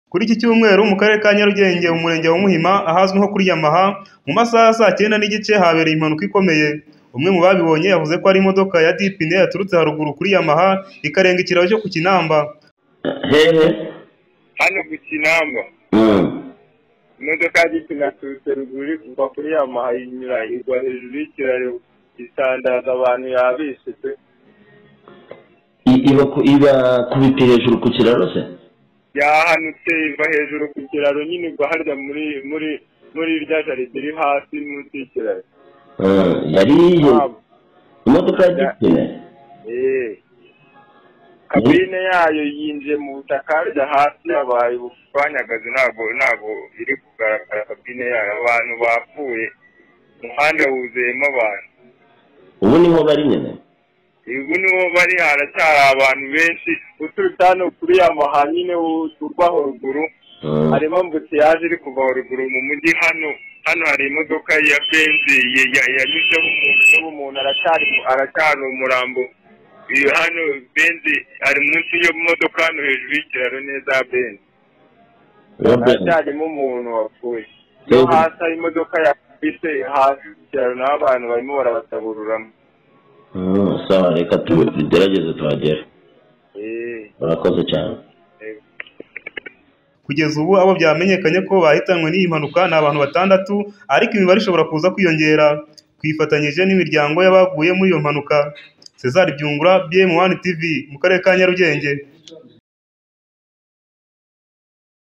🔴AMAKURU MASHYA ABABAJE KU RWANDA, 6 BARAPFUYE MU MUJYI WA KIGALI KU MUHIMA, IREBERE IYI NSANGANYA.
amakurumashyamurwanda ABABAJE KU RWANDA, 6 BARAPFUYE MU MUJYI WA KIGALI KU MUHIMA, IREBERE IYI NSANGANYA.